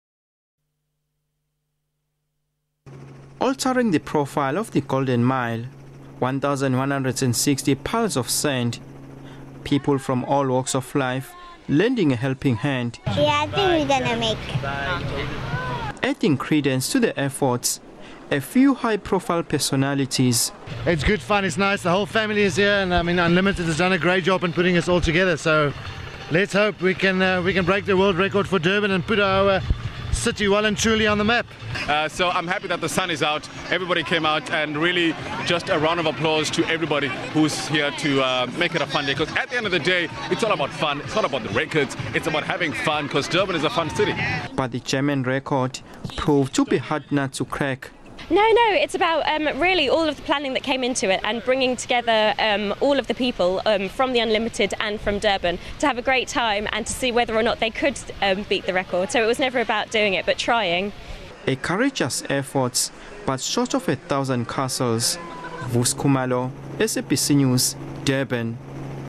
SA fails to breaks sandcastle Guinness World Record
South Africa has been crowned as the first country in Africa attempting to break the Guinness World Record for building the most sandcastles in one hour.More than 800 people joined in the fun at Durban's Bay of Plenty Beach. Only 1 600 sandcastles were built, which was just over half the current record.